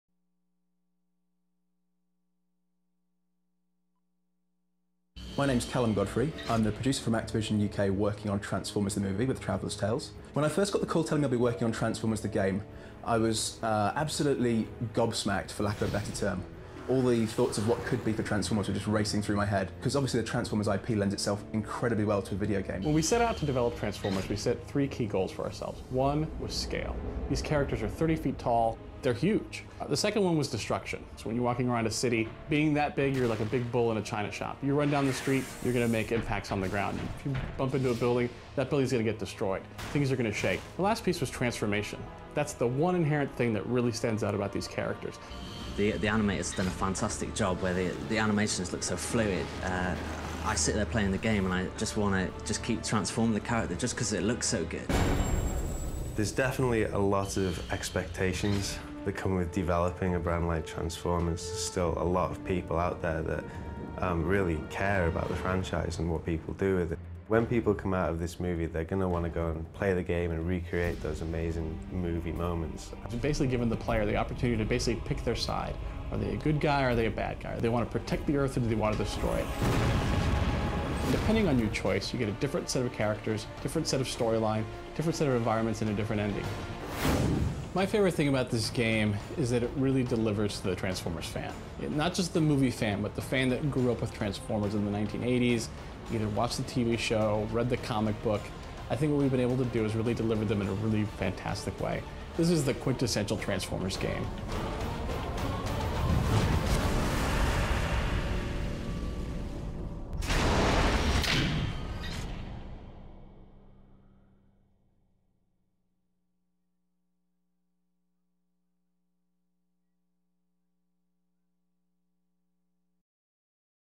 Transformers: The Game Video Interview
Transformers: The Game video interview featuring a behind-the-scenes look at the game. In it, members of the Traveller's Tales and Activision development teams discuss the game.  for more gaming news